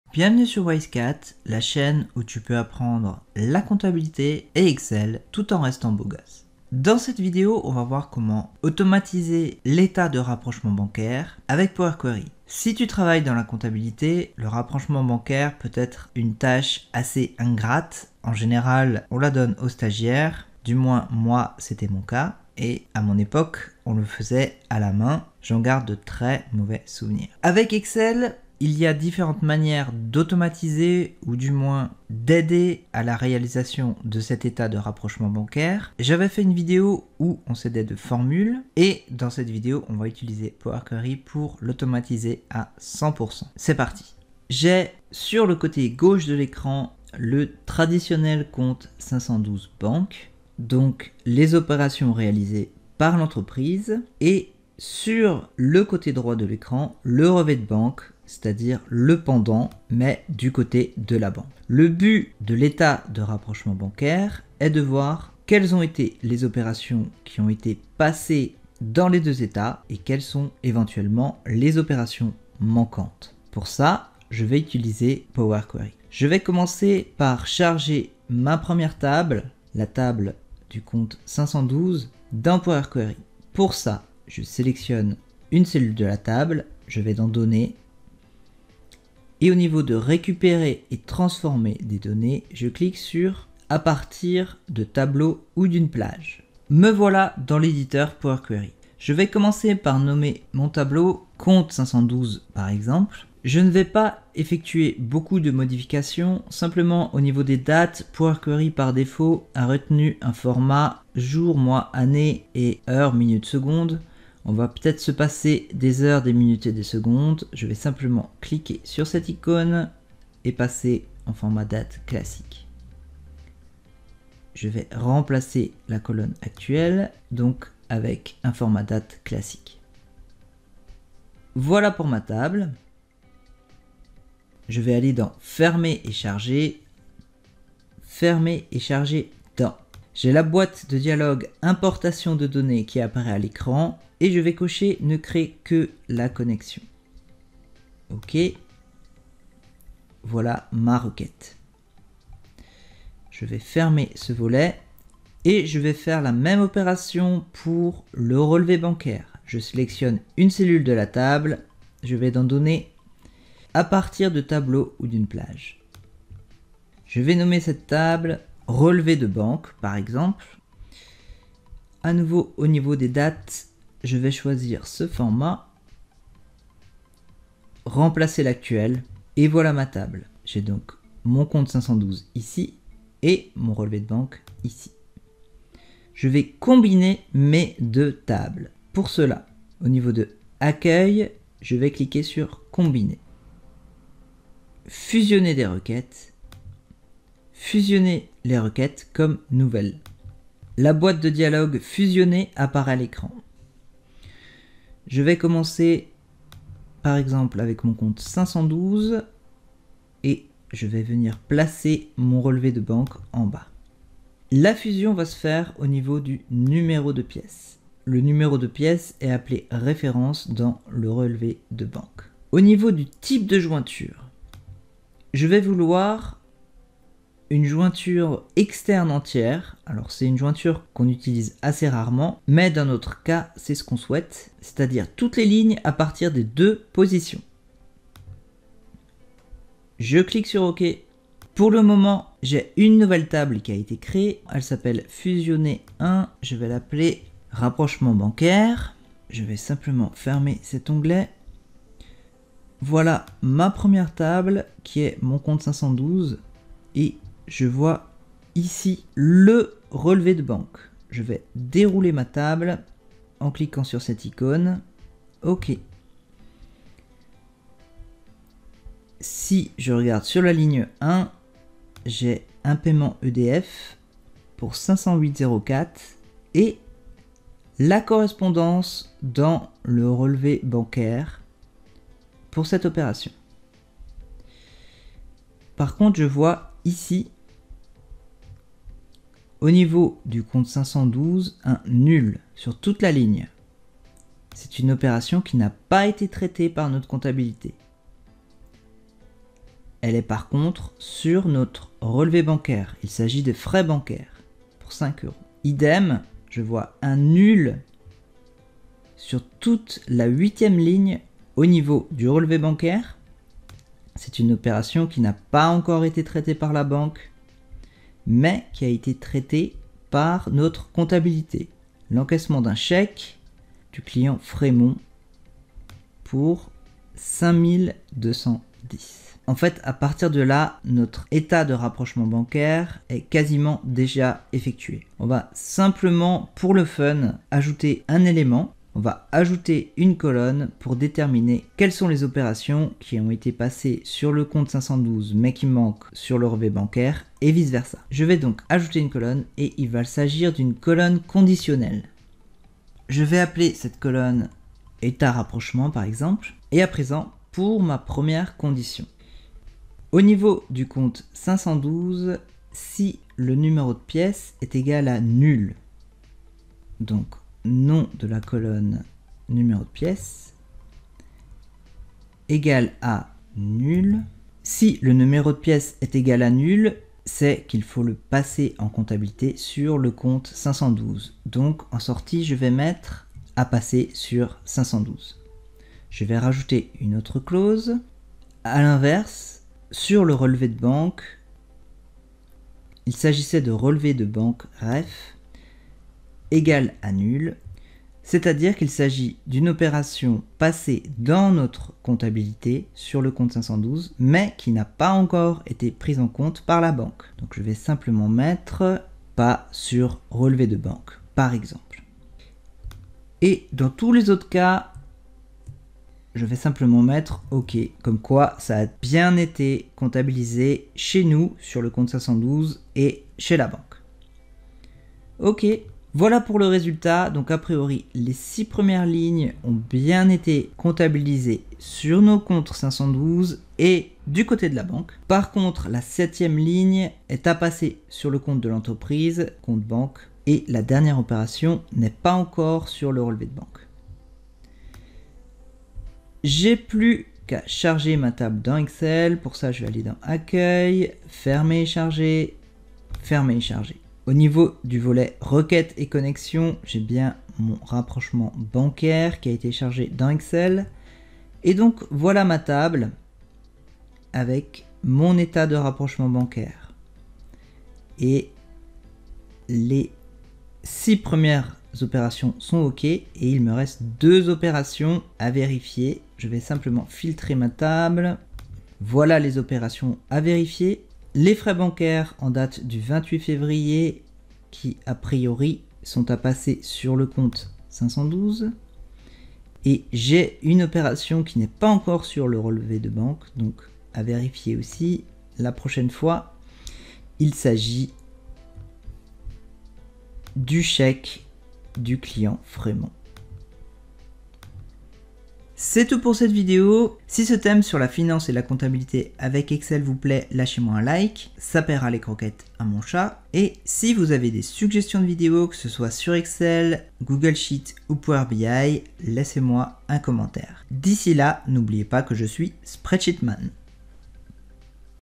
Comptabilité et finance avec Excel : Automatisez votre état de rapprochement bancaire (Power Query)
Découvrez comment automatiser un état de rapprochement bancaire avec Excel dans cette formation pratique pour la comptabilité et la finance. Utilisez Power Query pour améliorer vos compétences en automatisation financière.

Cette vidéo peut-être utile aux étudiant en BTS comptabilité, et au cursus de l'expertise comptable :  dcg, dscg et à toute personne qui s'intéresse à la finance.

Si mon travail vous intéresse, j'ai écrit plusieurs ouvrages publiés aux éditions Eni sur la business intelligence avec Excel (Power BI) :

Un tutoriel Excel et Power Query en français proposé par Wise Cat Noro Boris

00:00 introduction
01:15 charger les tables dans Power Query
03:05 combiner les requêtes
05:40 colonne conditionnelle
12:05 la jointure
14:17 le compte de resultat